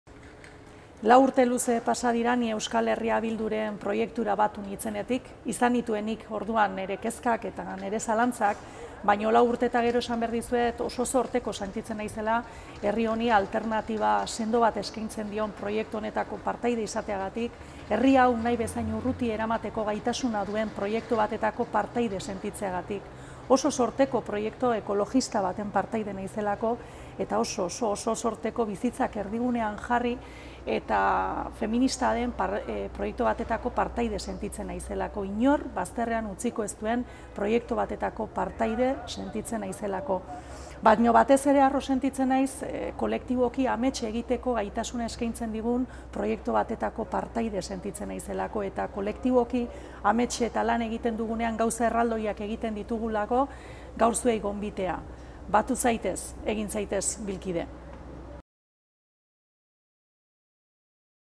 Nerea Kortajarena
Nerea Kortajarena, EH Bilduko legebiltzarkide eta Gasteizko Legebiltzar taldeko koordinatzaileak, Bilkide egiteko gonbita luzatzen dizue.